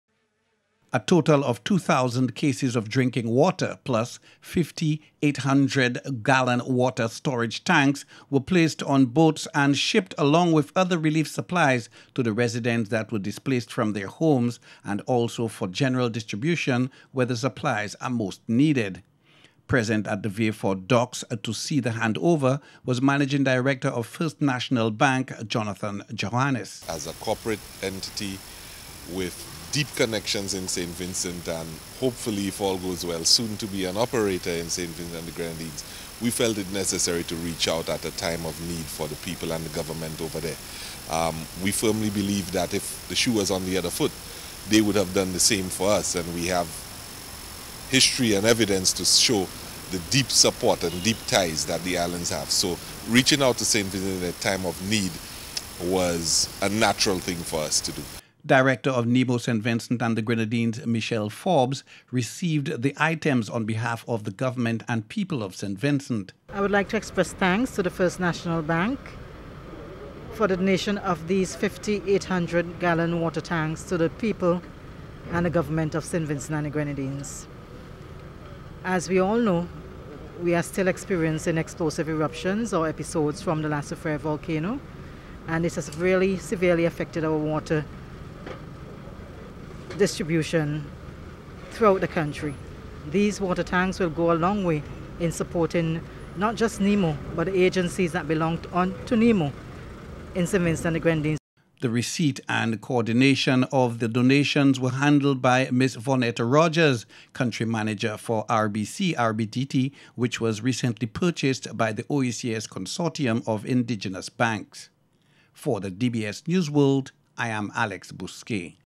First National Bank Assists Vincentians as Volcano Continues to Erupt
1st National Bank St. Lucia Limited has made a significant donation of drinking water and water storage tanks for the families most affected by the eruptions of the La Soufriere Volcano in sister island of St Vincent and The Grenadines.